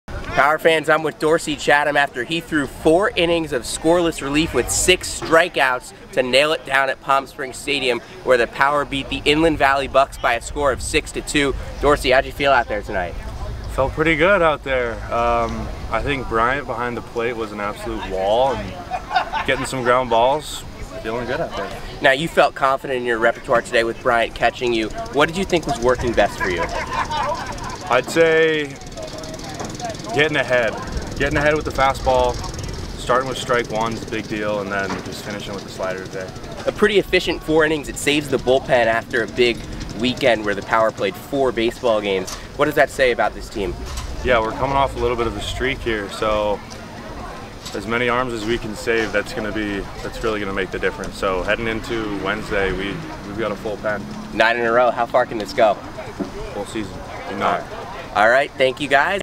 Palm Springs Power Post Game Interview: PS Power vs Inland Valley Bucs (06/13)
Dorsey Chatham took a minute to talk with broadcaster, Sam Federman, following the Power’s 6-2 win over the Inland Valley Bucs!